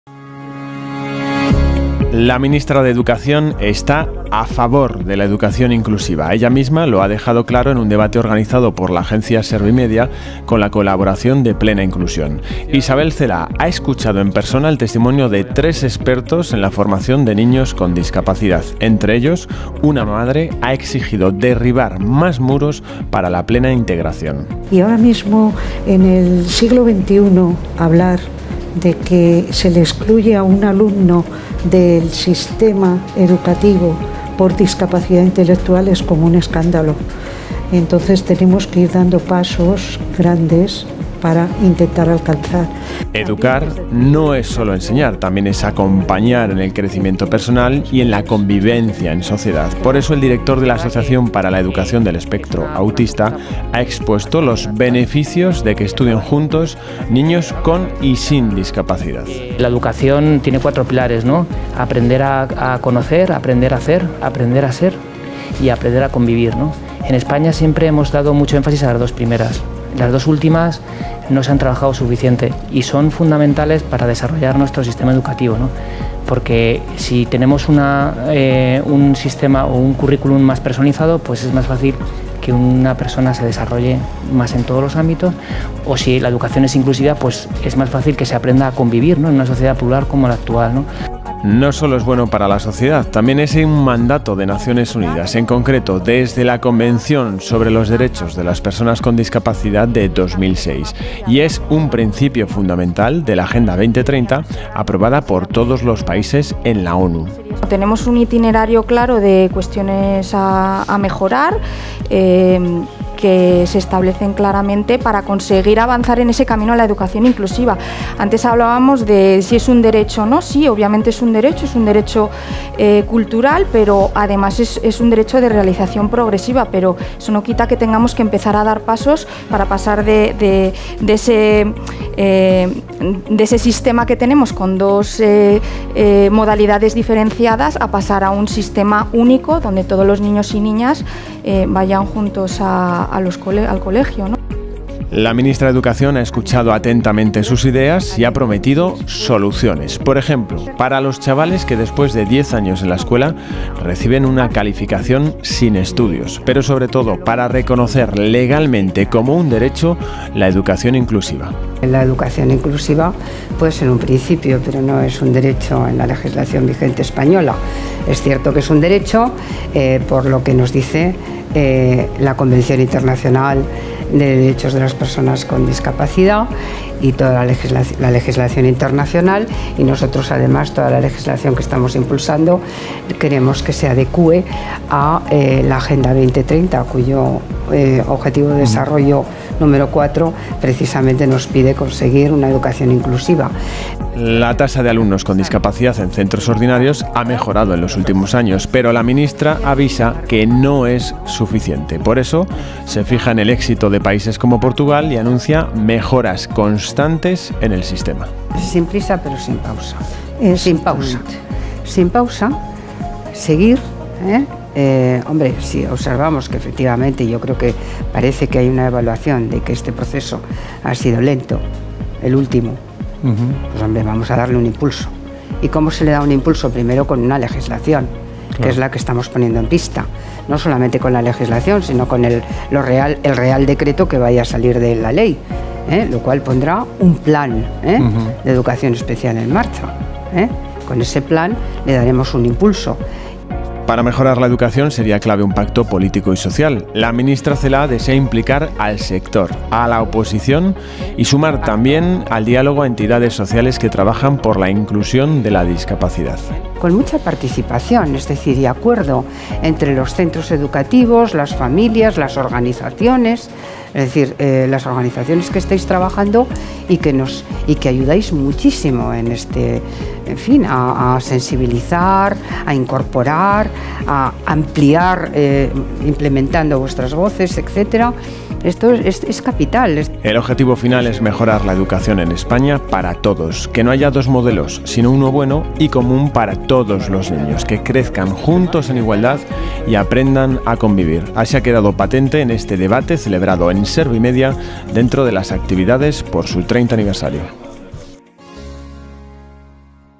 Encuentro inf Plena inclusión/Servimedia sobre Educación Inclusiva
Con la presencia de la ministra de Educación, Isabel Celaá, se celebró el 7 de mayo un encuentro informativo en Servimedia para hablar sobre educación inclusiva. También intervinieron Macu Blanco, madre de un chaval con discapacidad intelectual; Inés de Araoz, responsable del Área Jurídica de Plena inclusión España; y Pedro  del Río, director de Aleph-TEA. bajo la moderación de José Manuel gonzález Huesa, director de Servimedia.